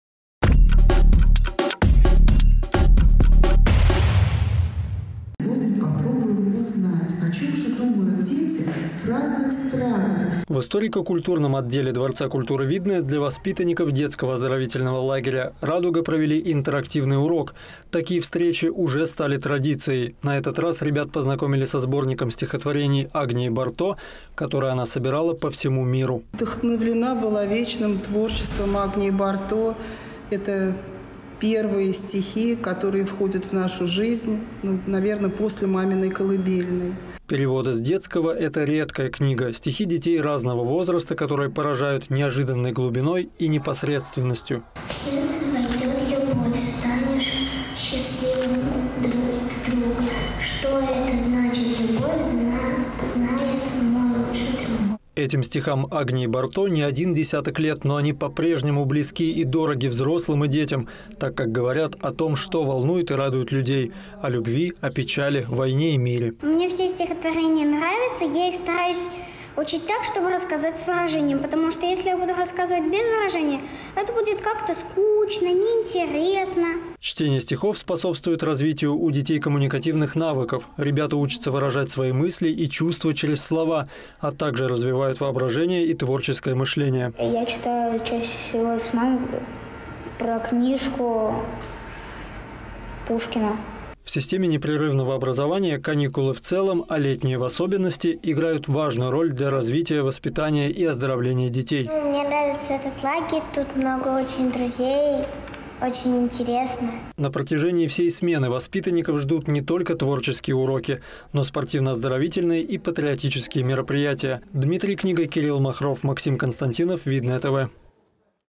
ПЕРЕВОДЫ С ДЕТСКОГО
Наша Таня громко плачет: уронила в речку мячик. Эти строки из стихотворения Агнии Барто известны каждому ребенку. Ребята из лагеря «Радуга» познакомились со сборником «Переводы с детского»,  в котором знаменитая поэтесса перевела стихотворения со многих языков мира.